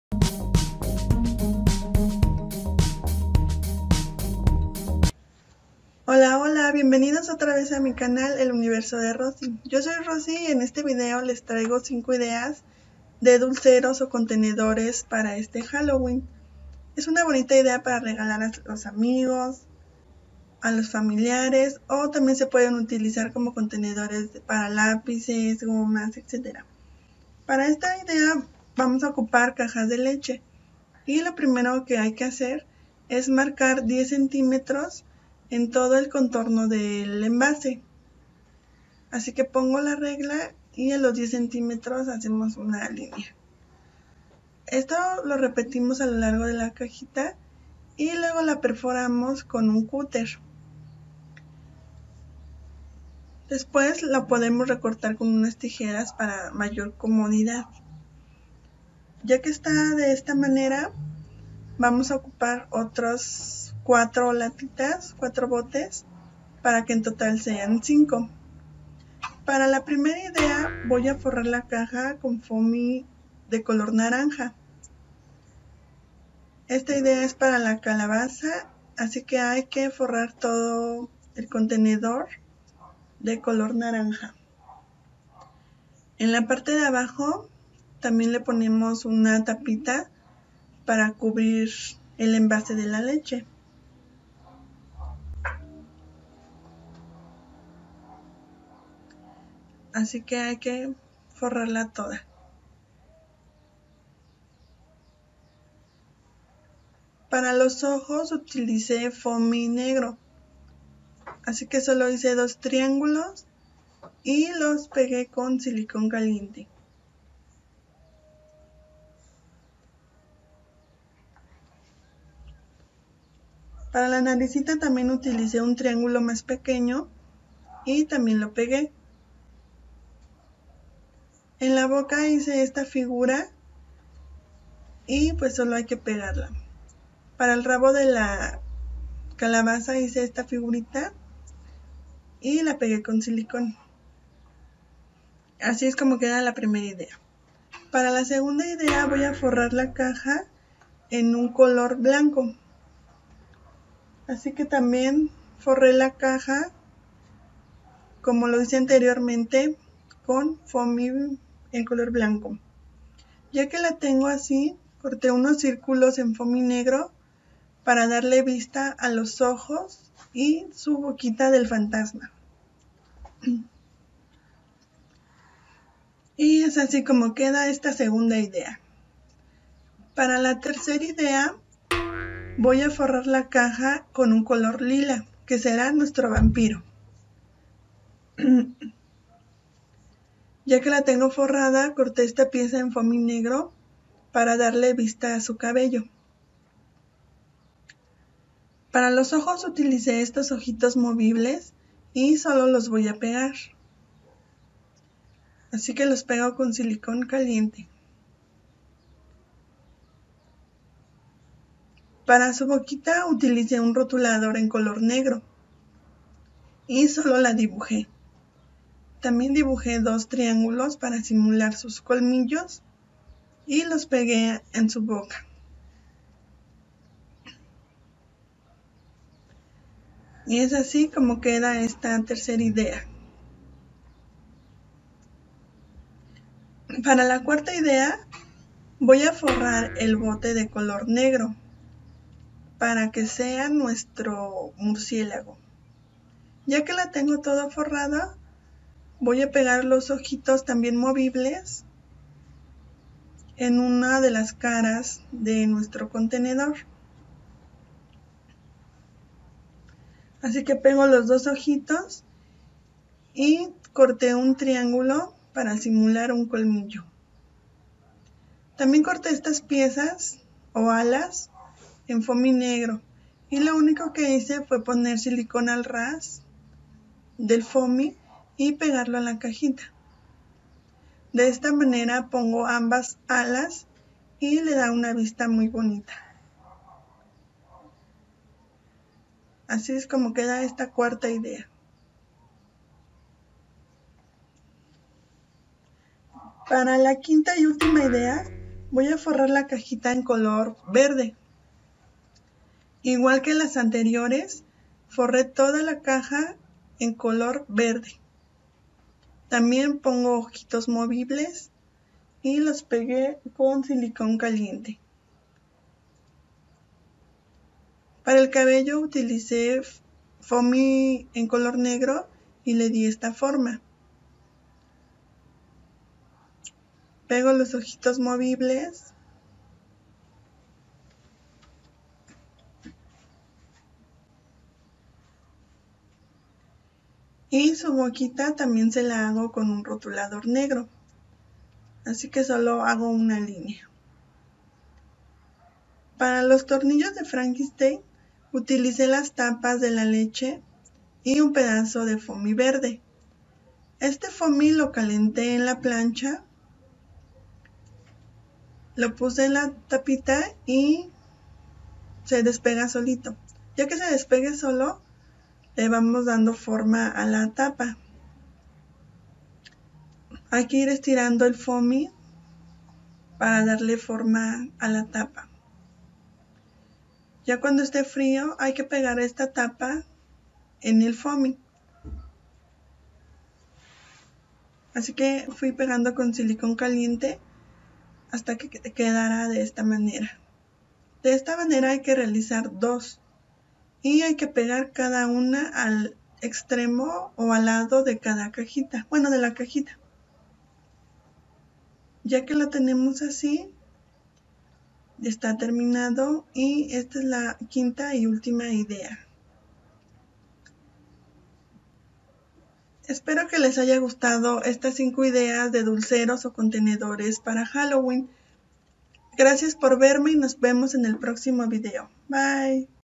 DULCEROS/CONTENEDORES PARA REGALAR EN HALLOWEEN // El Universo De Rosy
Hola chicos y chicas en este video les muestro cinco ideas para hacer dulceros con el tema de halloween. Espero les guste y gracias por verme.

- Activar la campanita de notificaciones para que no perderte ningún video

- Dejarme tu comentario

Gracias y nos vemos en el próximo video XD
XOXOXO